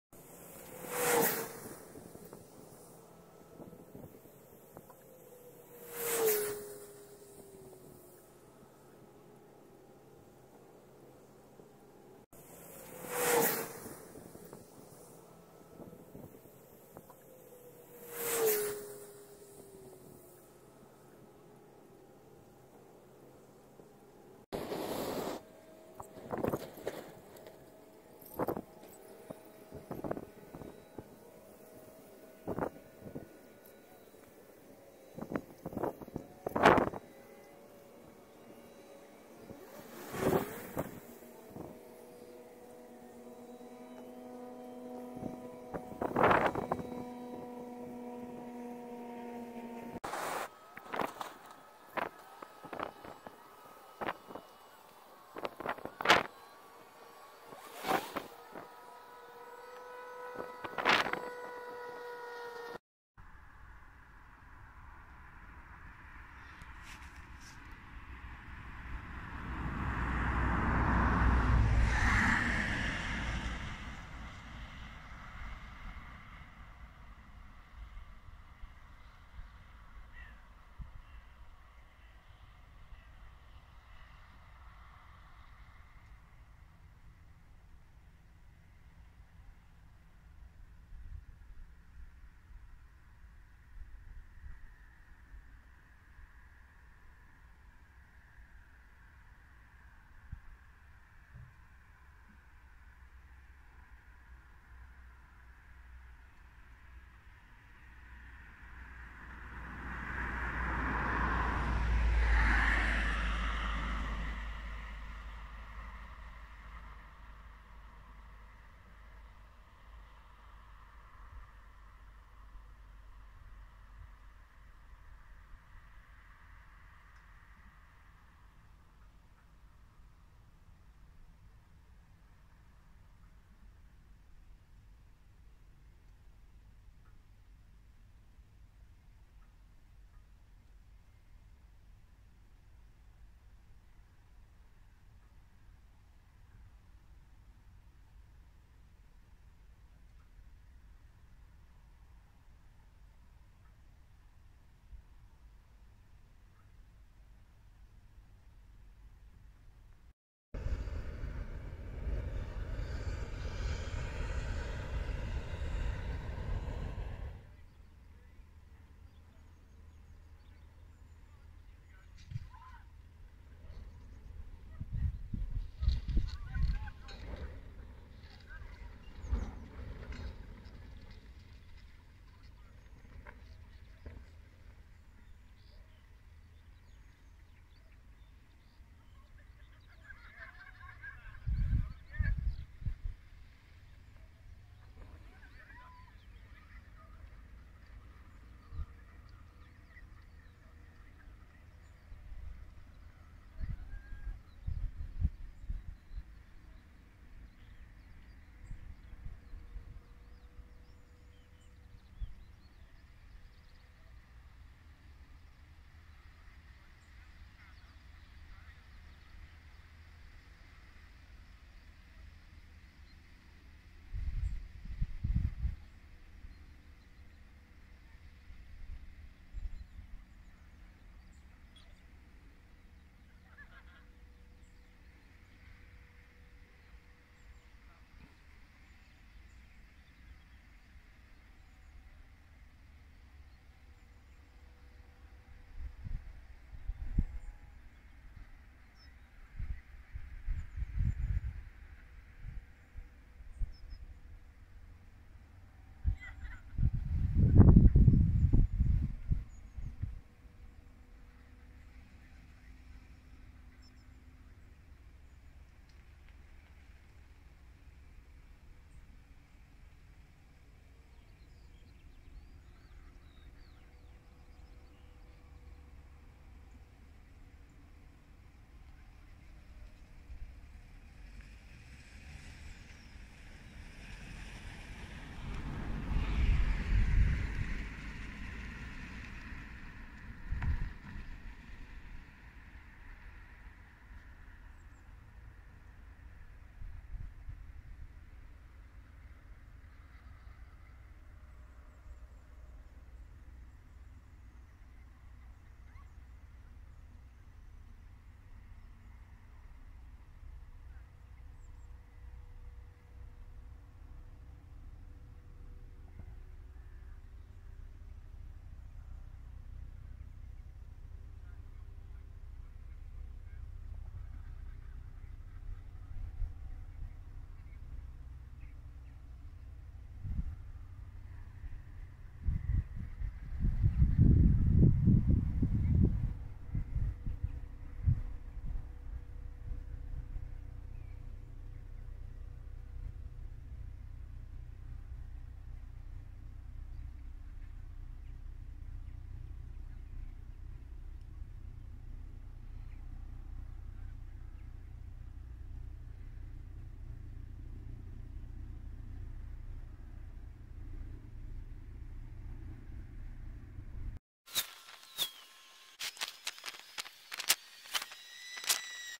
Hot air balloon crash
UK hot air balloon crash.
Cameron A-400 G-VBAP.
16th October 2021.